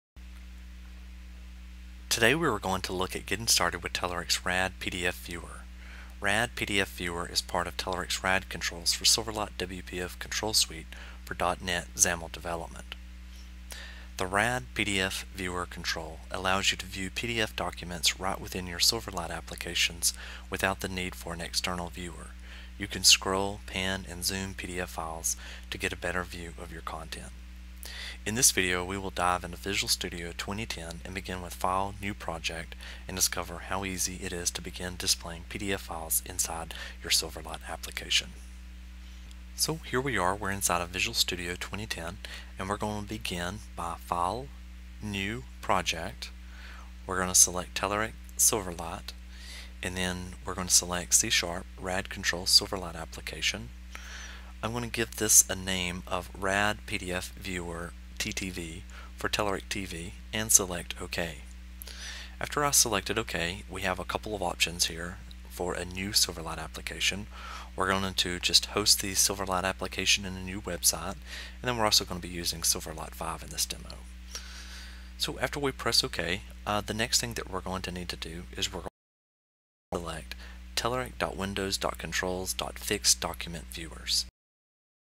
PDFViewer- Part 1: Getting Started (Silverlight & WPF)
In this video, we will dive into Visual Studio 2010 and discover how easy it is to begin displaying PDF Files inside the RadPDFViewer control.